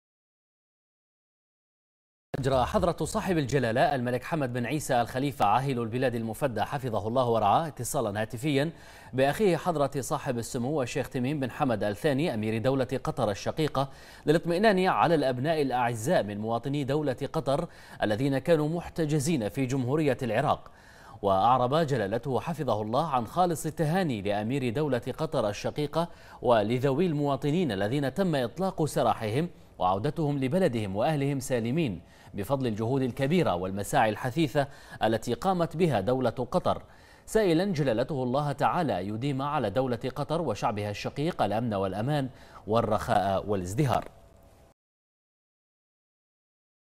البحرين: عاهل البلاد المفدى يجري إتصالاً هاتفياً مع سمو أمير دولة قطر
المنامة في 21 ابريل / بنا / أجرى حضرة صاحب الجلالة الملك حمد بن عيسى آل خليفة عاهل البلاد المفدى حفظه الله ورعاه اليوم اتصالا هاتفيًا بأخيه حضرة صاحب السمو الشيخ تميم بن حمد آل ثاني أمير دولة قطر الشقيقة للاطمئنان على الابناء الأعزاء من مواطني دولة قطر الذين كانوا محتجزين في جمهورية العراق.